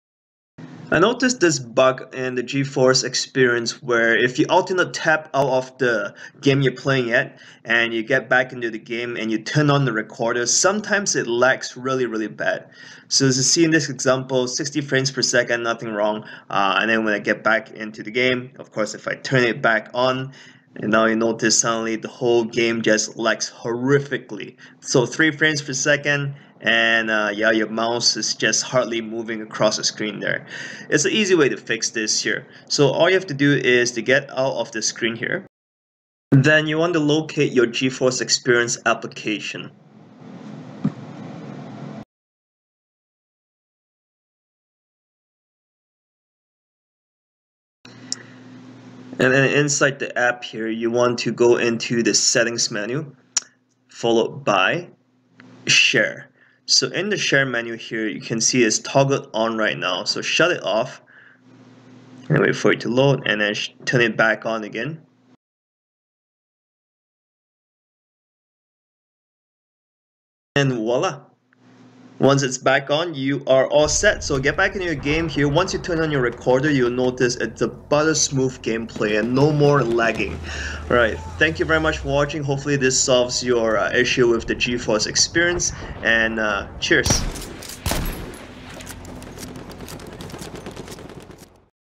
Low FPS & Not Recording Fix for Geforce Experience Shadowplay
Intermittent low FPS, stuttering, and not recording issue when Geforce Share (aka Shadowplay) recorder running? Now you can fix without resetting computer

This happens if you alt-tab often and start up recorder again only to experience extremely low frame rates. Do not need to restart your computer just to make it work anymore and will save you a lot of time especially if you are a streamer. Hope this works for you!